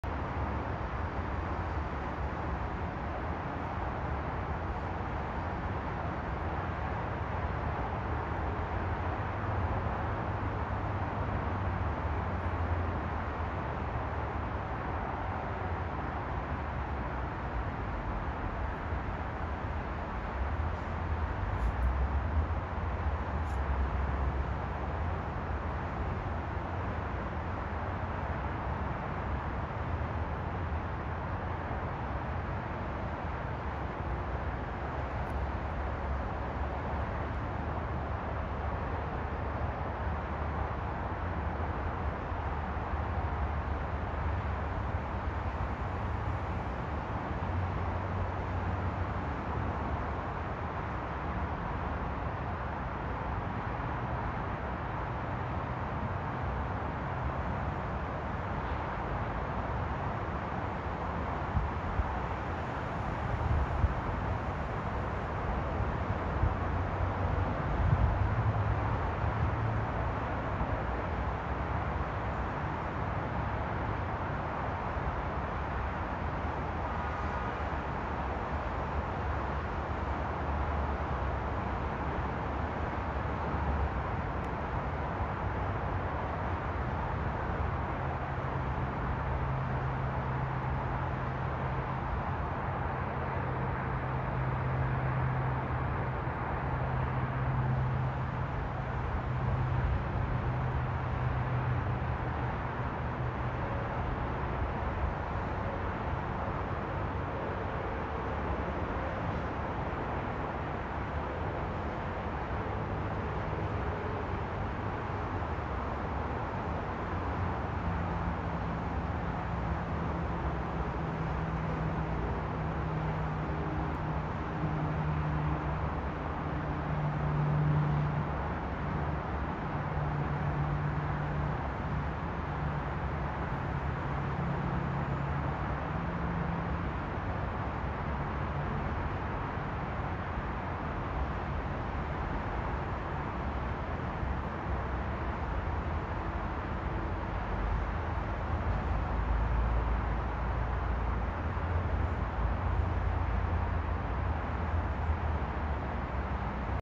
Rocket Launch over San Diego. September 14, 2023
An Alpha rocket launched from Vandenberg Space Force Base by the startup company Firefly Aerospace lit up the sky over coastal San Diego County just after sunset on Thursday.

The 97-foot-long rocket’s first stage left a corkscrew-shaped contrail, while the second stage could clearly be seen heading south high in the western sky.